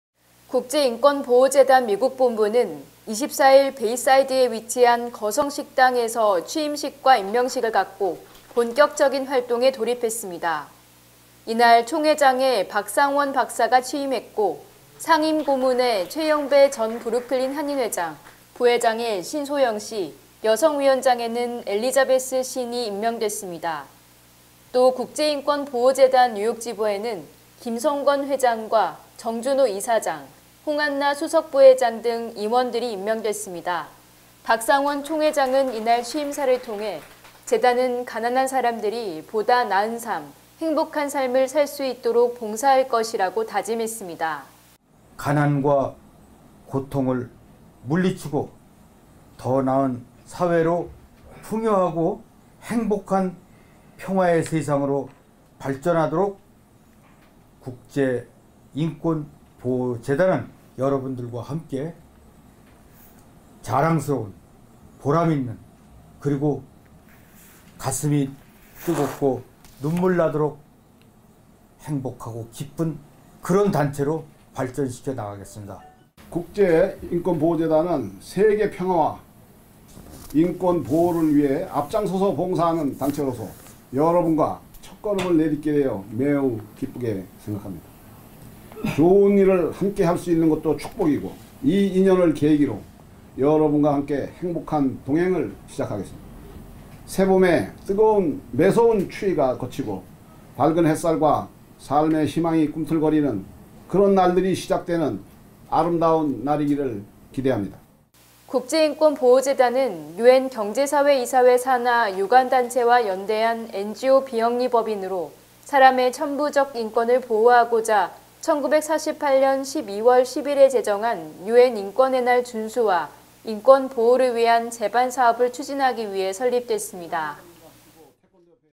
[LOCAL NEWS] 국제인권보호재단 미국본부 발족
총회장에 박상원 박사, 최영배 상임고문 임명

미 전역 가난과 차별을 없애도 인권을 보장하기 위핸 활동을 설립된 국제인권보호재단 미국본부가 공식적으로 출범했습니다.

국제인권보호재단 미국본부는 24일 베이사이드에 위치한 거성 식당에서 취임식과 임명식을 갖고 본격적인 활동에 돌입했습니다. 이날 총회장에 박상원 박사가 취임했고 상임 고문에 최영배 전 브루클린한인회장, 부회장에 신소영씨, 여성위원장에는 엘리자베스 신 이 임명됐습니다. 또 국제인권보호재단 뉴욕지부에는 김성권 회장과 정준호 이사장, 홍한나 수석부회장 등 임원들이 임명됐습니다. 박상원 총회장은 이날 취임사를 통해 재단은 가난한 사람들이 보다 나은 삶, 행복한 삶을 살수 있도록 봉사할 것이라고 다짐했습니다.

국제인권보호재단은 UN 경제사회이사회 산하 유관단체와 연대한 NGO 비영리 법인으로 사람의 천부적 인권을 보호하고자 1948년 12월 10일에 제정한 UN 인권의 날 준수와 인권보호를 위한 제반 사업을 추진하기 위해 설립됐습니다.